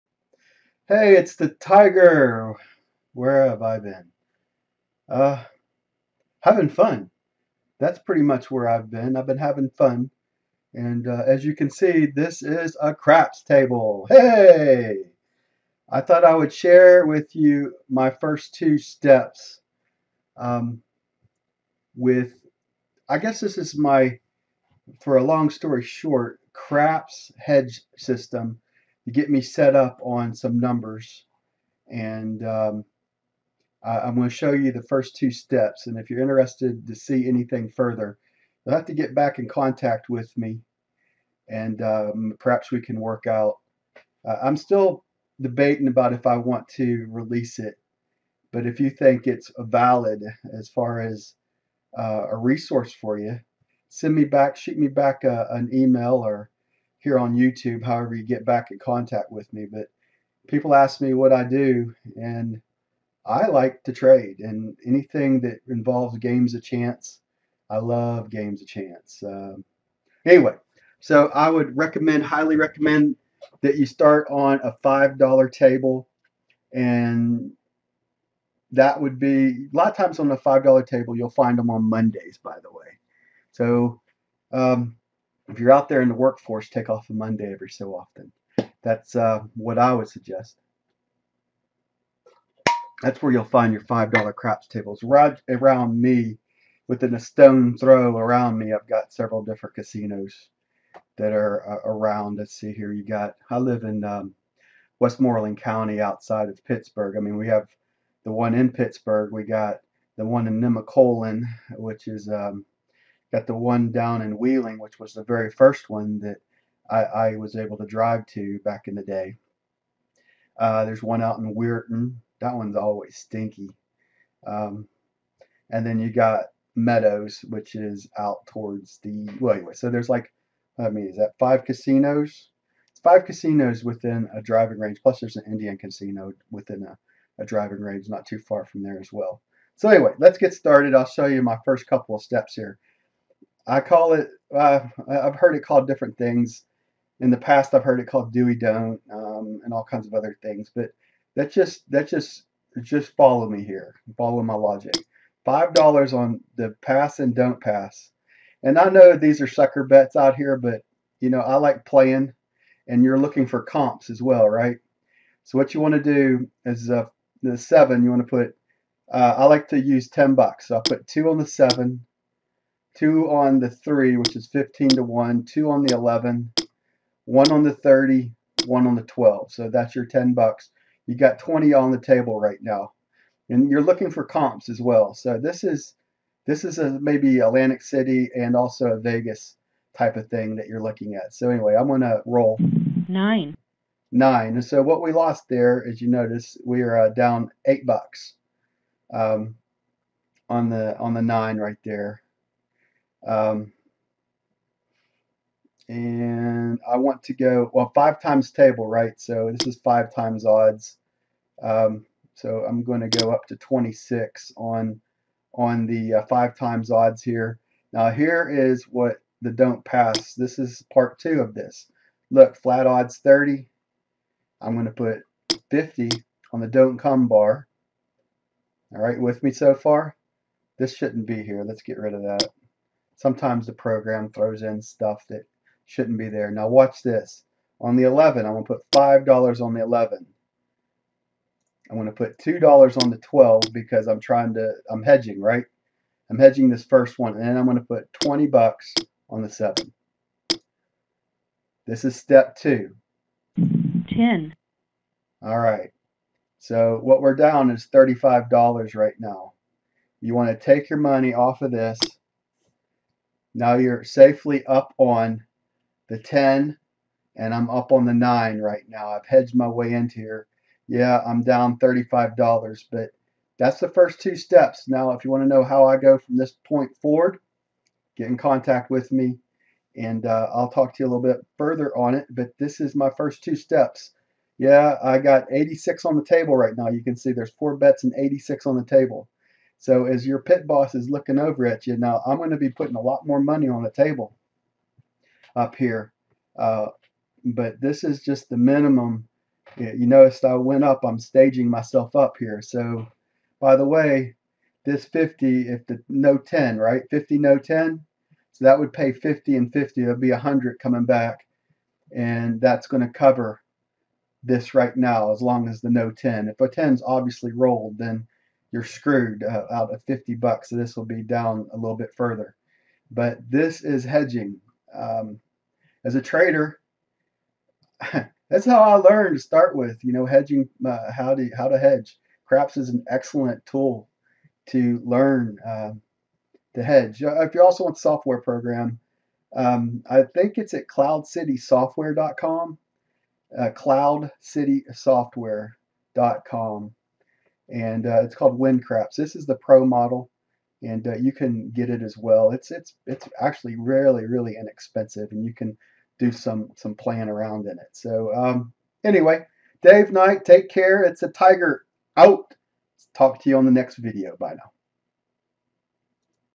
Craps Hedge Strategy
If you are watching this video without being inside my playlist ... I have a bunch of casino related videos on a playlist.

More information to come a little later ... this video shows you my first two steps to my casino craps hedge strategy.

Stay Cool,

If you like this video or don't ... here is another video with a more conservative approach for a smaller bankroll: